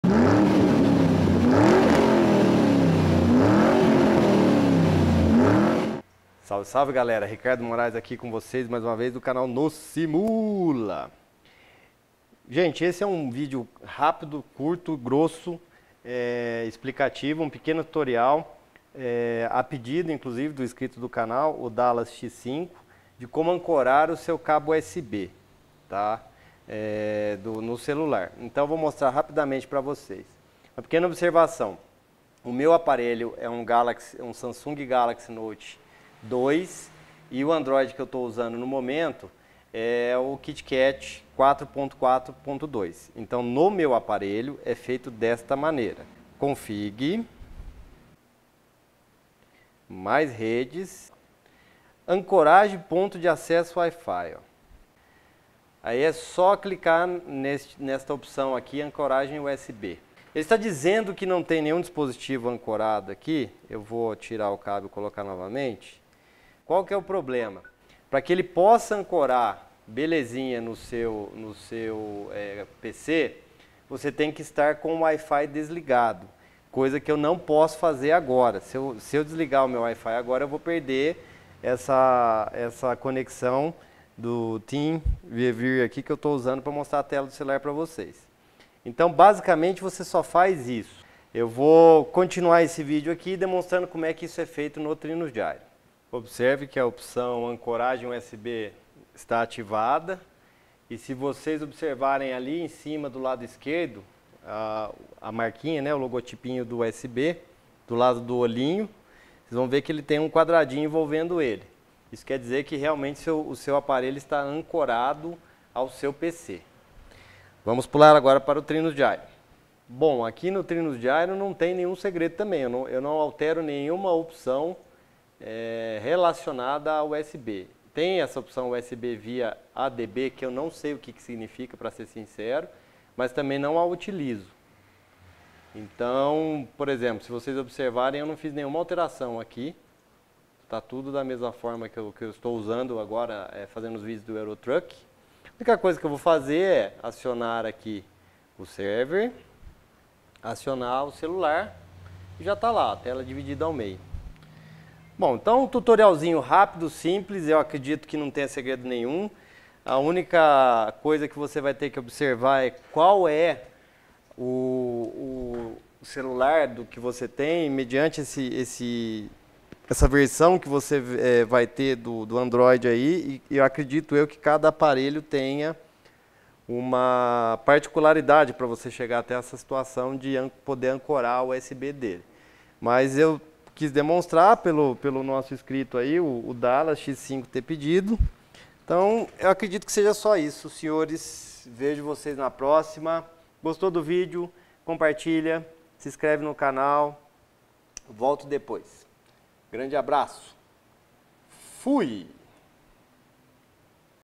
Como Ancorar seu Smartphone no PC + Trinus Gyre
Salve galera, neste pequeno vídeo demonstro como ancorar o smartphone no PC e algumas informações sobre o Trinus Gyre funcionando com este sistema.